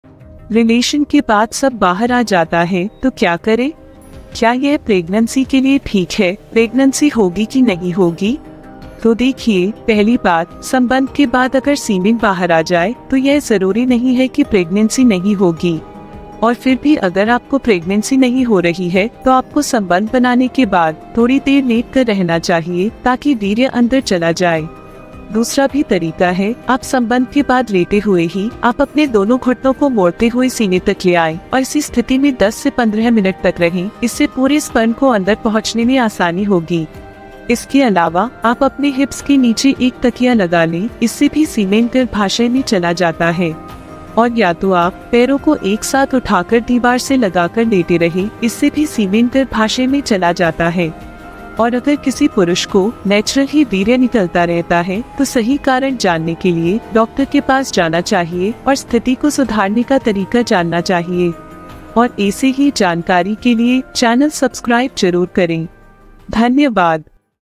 RELATION के बाद सब बाहर आ जाता है, तो क्या करे? सम्बन्ध कैसे बनाएं?
RELATION के बाद सब बाहर आ जाता है, तो क्या करे?
क्या यह प्रेग्नन्सी के लिए ठीक है? प्रेग्नन्सी होगी की नहीं होगी ?
तो देखिए,
पहली बात, संबंध के बाद अगर सीमेन बाहर आ जाए, तो यह ज़रूरी नहीं है कि प्रेग्नेंसी नहीं  होगी।
और फिर भी  अगर आपको प्रेग्नेंसी नहीं हो रही है, तो आपको संबंध बनाने के बाद, थोड़ी देर लेट कर रहना चाहिए, ताकि वीर्य अंदर चला जाए.
दूसरा भी तरीका है  आप संबंध के बाद  लेटे  हुए ही आप  अपने दोनों घुटनों को मोड़ते हुए सीने तक ले आएं और इसी स्थिति में दस  से पंद्रह  मिनट तक रहें. इससे पूरे  स्पर्म को  अंदर पहुंचने  में आसानी होगी.
 इसके अलावा, आप अपने हिप्स के नीचे एक तकिया लगा लें, इससे भी  सीमेन को गर्भाशय मे चला जाता है
 और या तो आप पैरों को एक साथ उठाकर दीवार से लगाकर लेटे  रहे, इससे भी  सीमेन को गर्भाशय मे चला जाता है
और अगर किसी पुरुष को नैच्रल ही  वीर्य निकलता रहता  है, तो सही कारण जानने के लिए डॉक्टर के पास जाना चाहिए,  और स्थिति को सुधारने का तरीका जानना चाहिए।
धन्यवाद